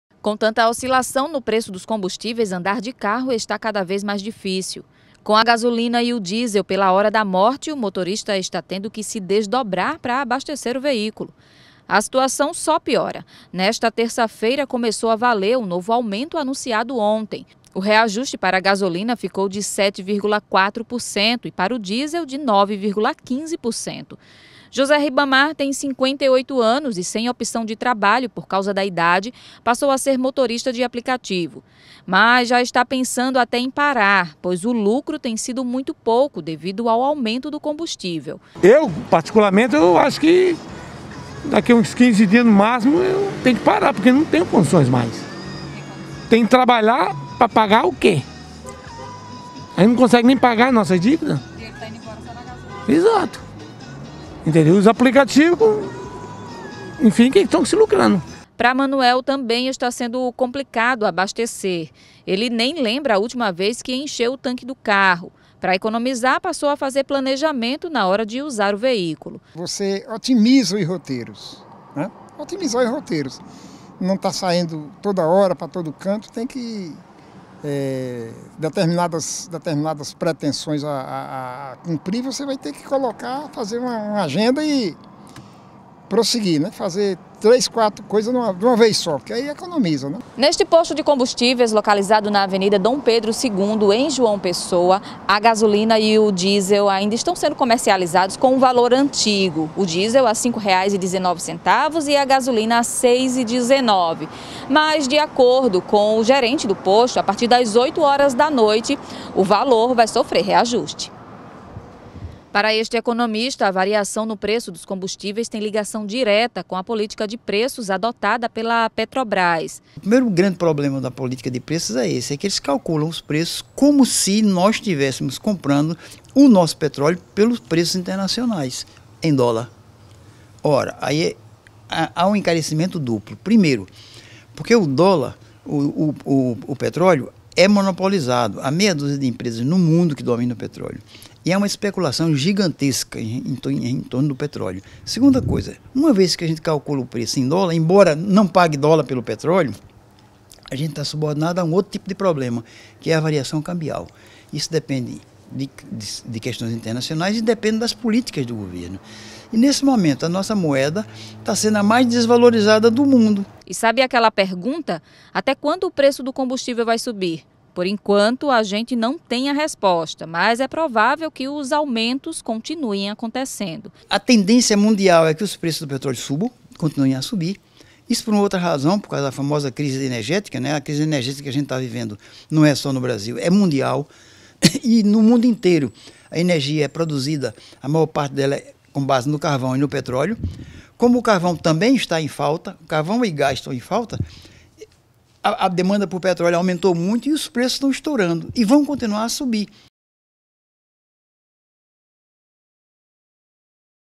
É só no nosso!!! Mais um aumento no valor dos combustíveis...
A Petrobras reajustou mais uma vez os preços da gasolina e do diesel para as distribuidoras. Segundo o comunicado divulgado nesta segunda-feira (25) pela petroleira, os novos valores passam a vigorar hoje (26). Joana Brito traz as informações.